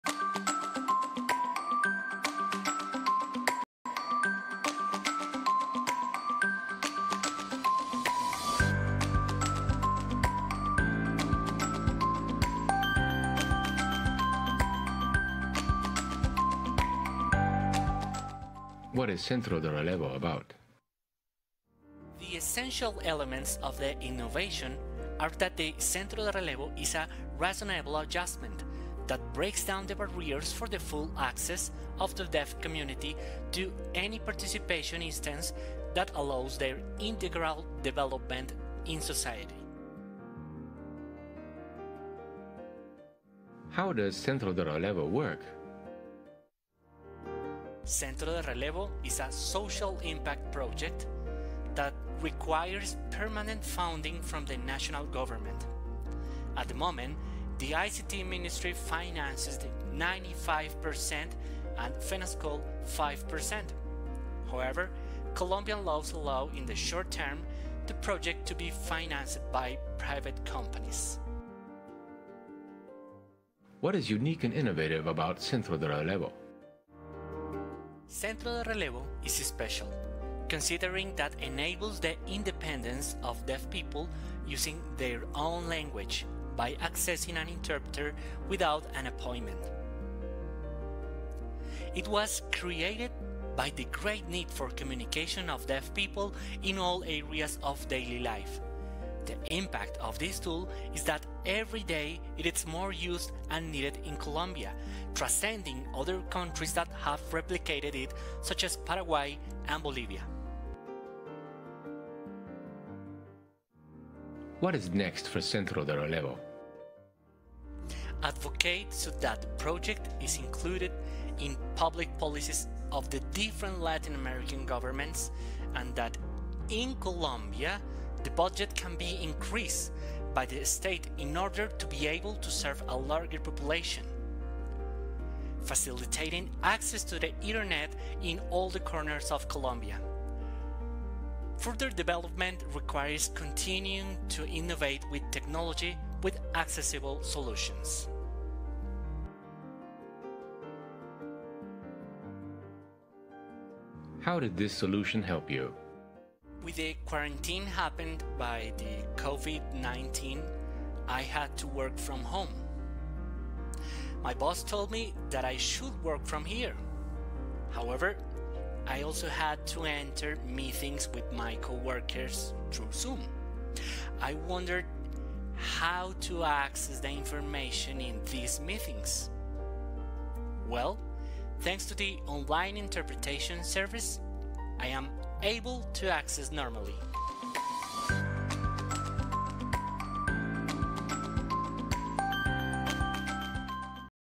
Fenascol en Zero Project
En la Conferencia @ZeroProjectLat  2020 la tématica se centró en Empleo y TIC.

Les compartimos la participación de @RoyetMejia quien habló de la experiencia de @centroderelevo, un caso de éxito de las TIC al servicio de las PS.

¡Somos ejemplo mundial!